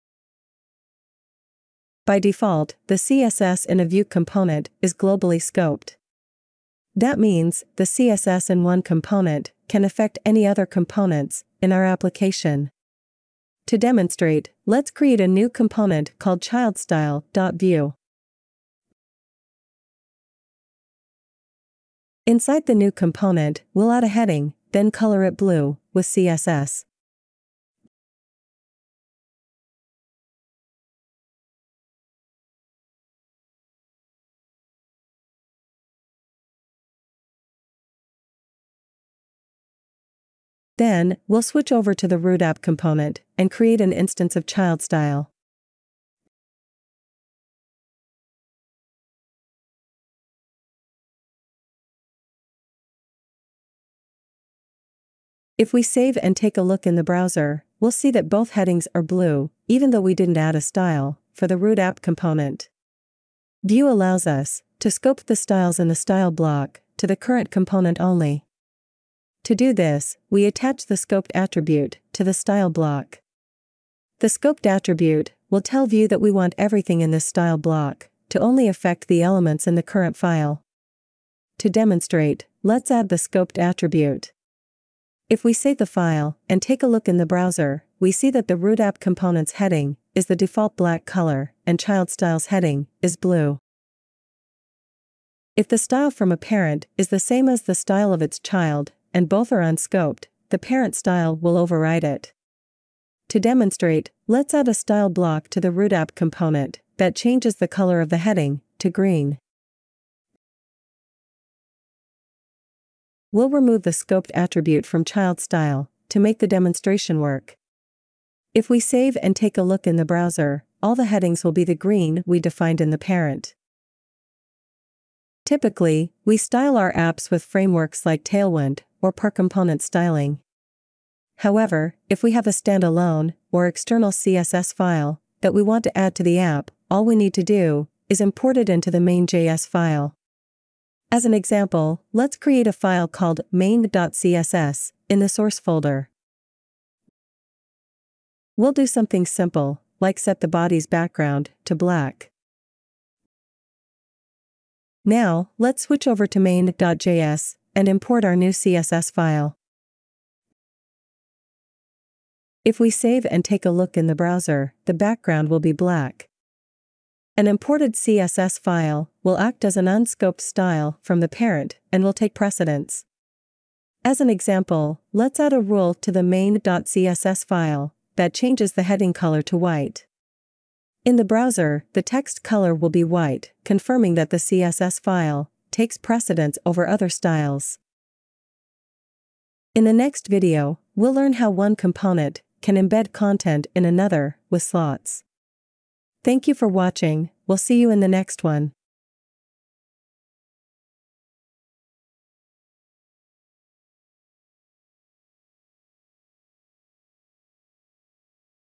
#17 - Component Styling - Vue 3 (Options API) Tutorial
In this Vue 3 tutorial, we learn how to style components. We cover scoping (local and global) and how to add external CSS files to your Vue application.

We cover the following topics:
1. Scoped styles: global and local
2. Unscoped parent style takes precedence
3. How to add standalone/external CSS file to a Vue project

_Note that this tutorial is for the Vue 3 Options API (which is similar to Vue 2). We later move on to the Composition API and then the Script Setup (3.2 update)_